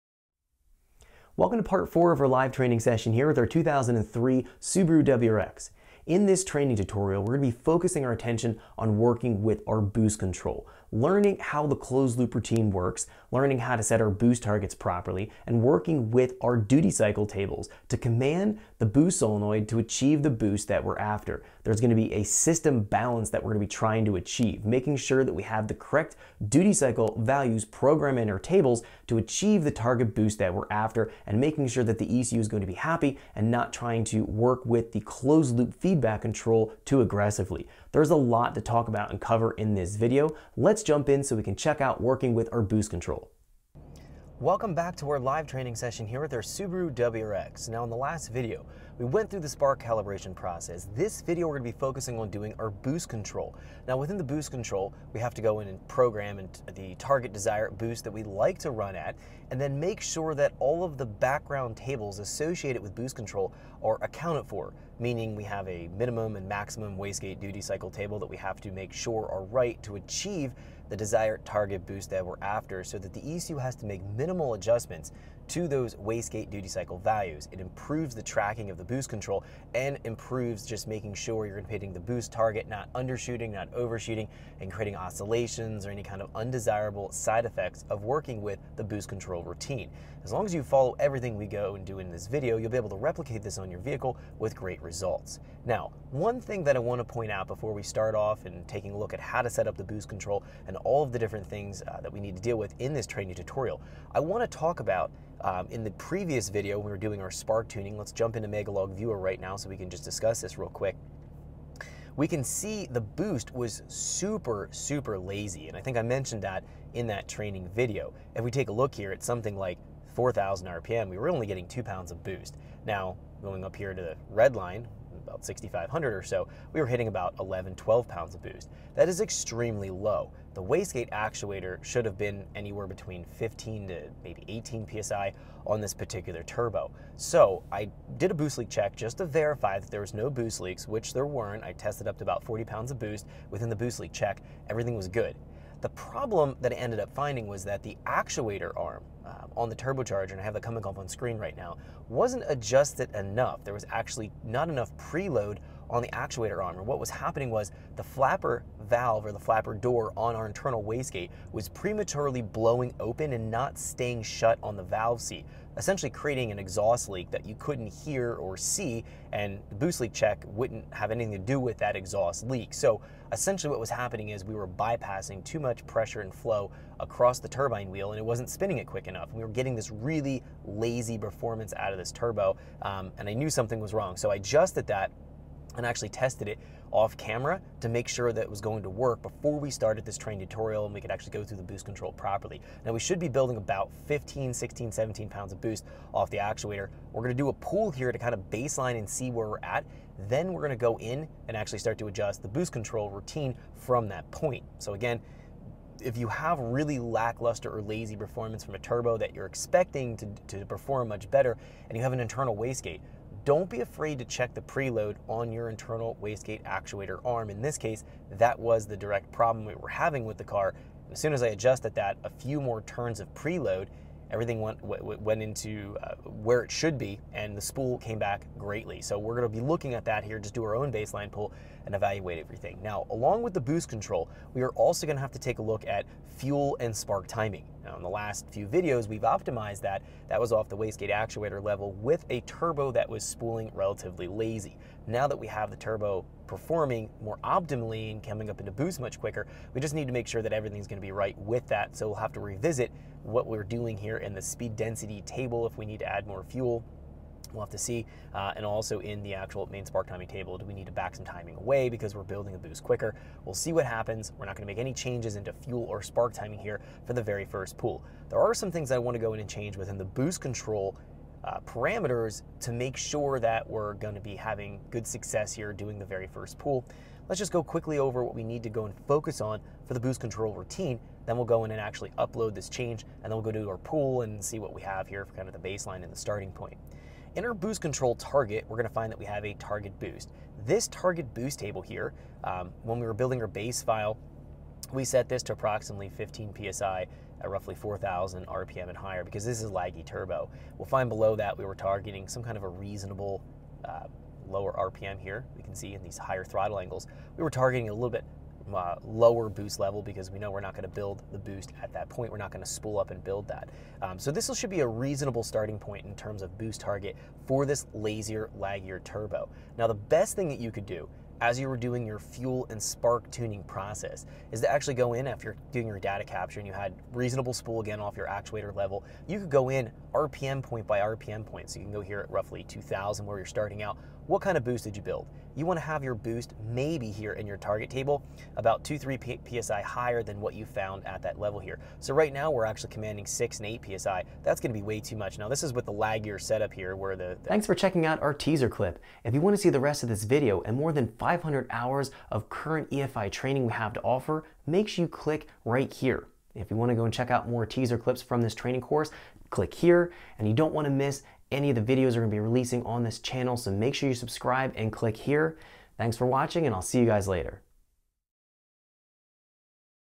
Ecu Flash 16 Bit Subaru Live Training Pt 4: Boost Control Tuning Routine | Evans Performance Academy
Ecu Flash 16 Bit Subaru Live Training Part 4: Boost Control Tuning Routine walks you through the entire closed-loop boost control tuning routine on a EJ20 engine.  In addition to the boost control tuning we also ensure proper fuel/spark timing under boost to complete the full throttle calibration process.